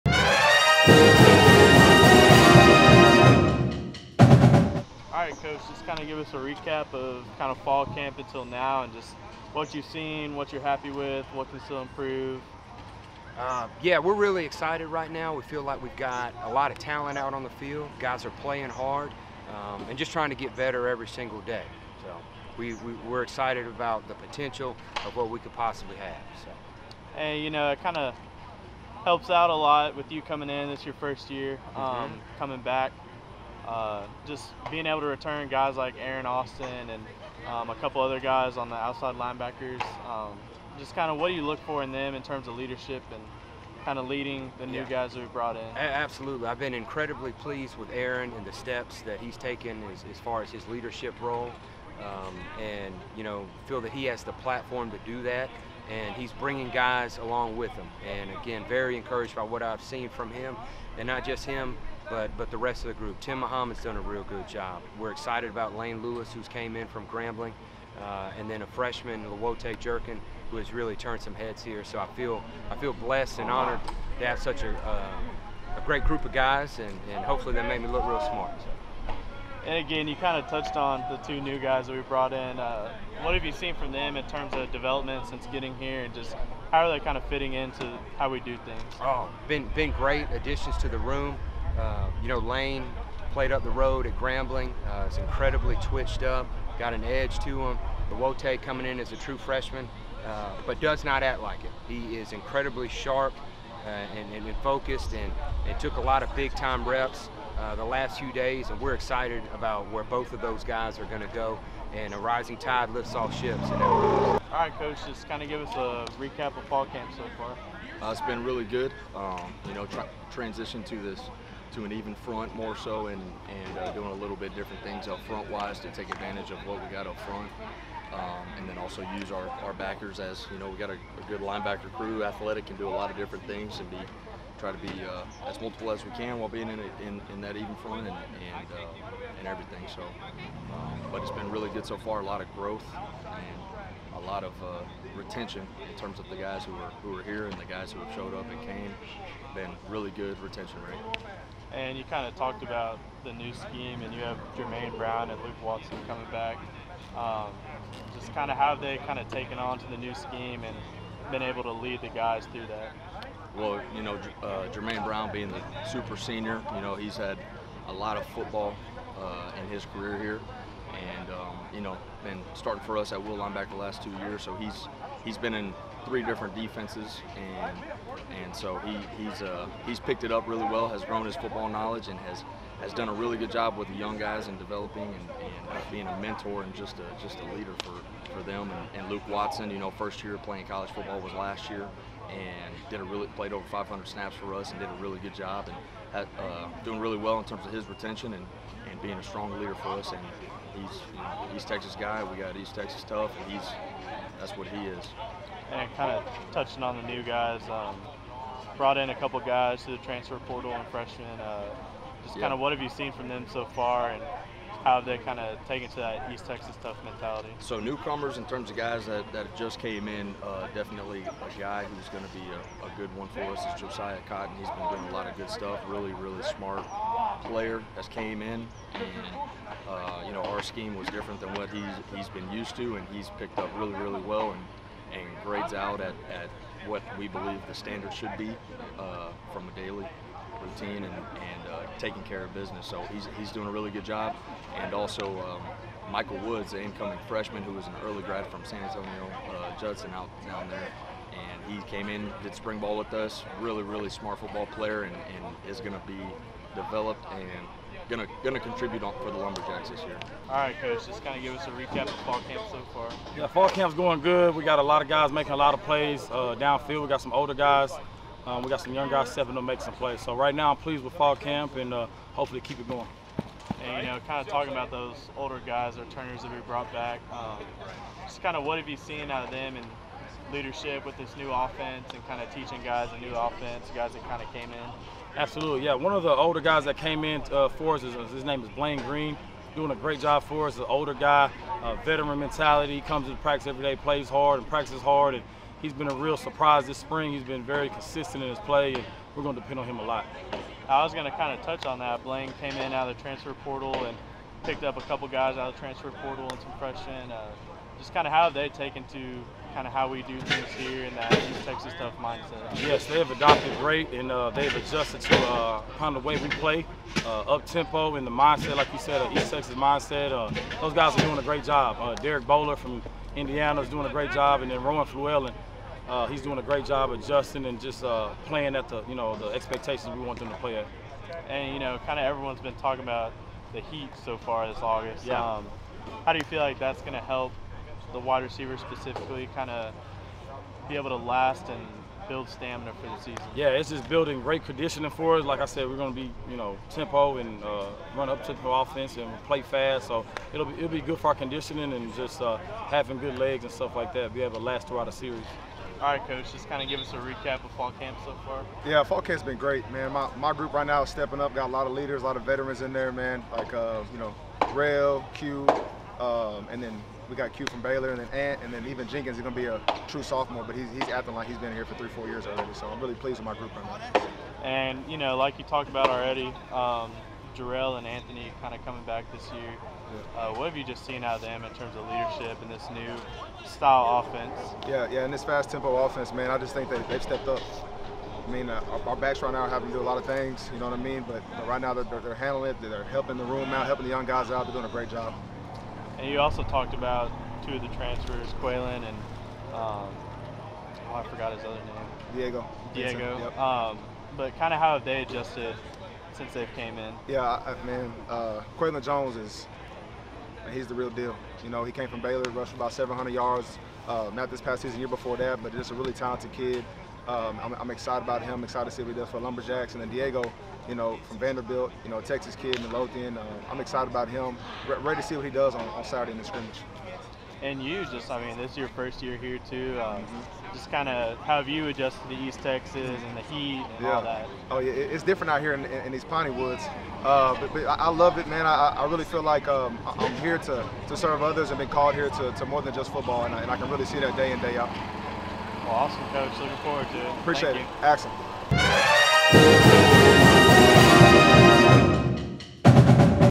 8.12.24 | 2024 Football Fall Camp Press Conference
Coaches Mackie, Wilson, Rogers and Martinez talk about what they have seen from their position groups so far in fall camp.